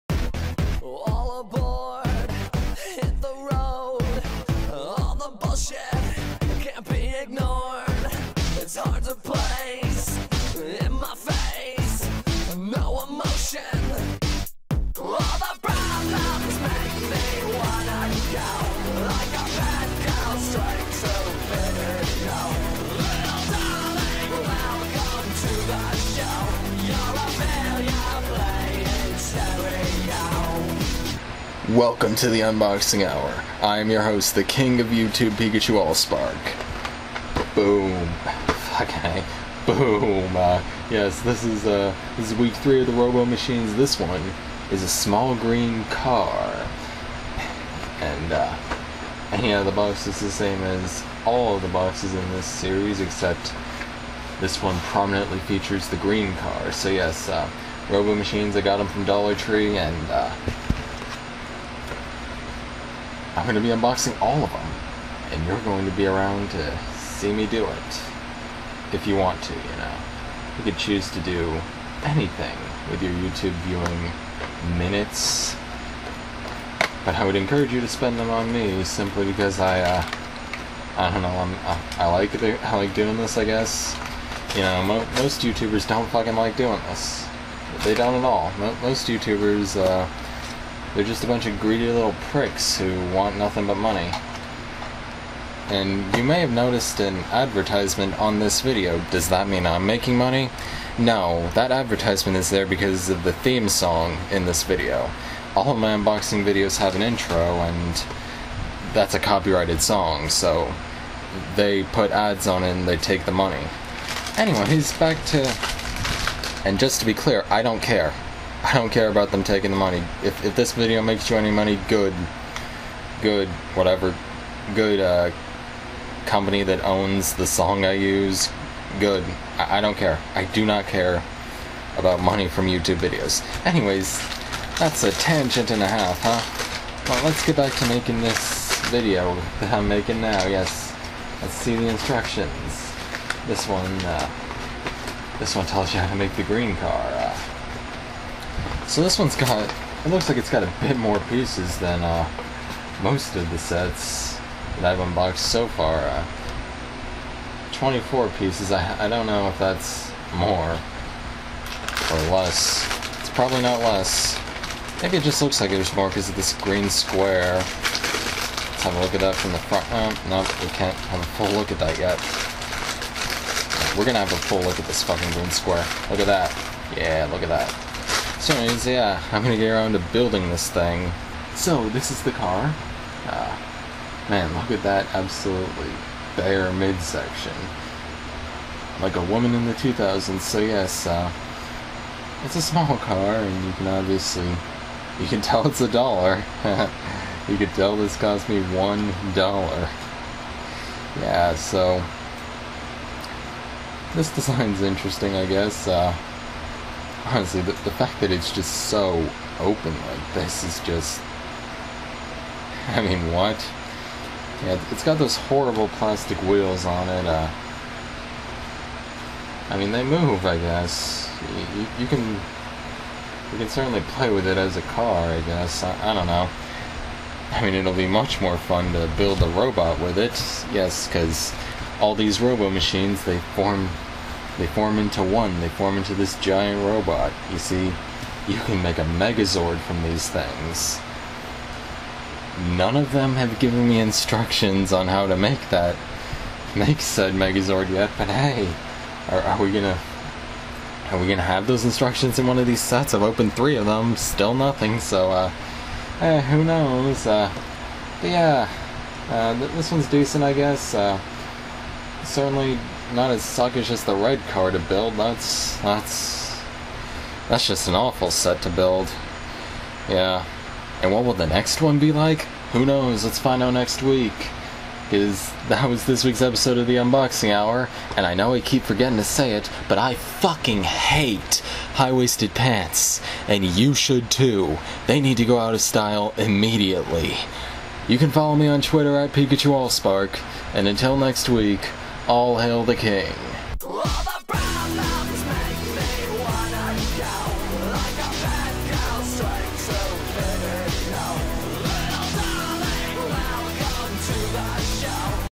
The Unboxing Hour: Block Tech Robo Machines 3
IT'S THE 3RD ONE OF THESE! Will I actually like this one? I don't know but that robot's gonna be cool.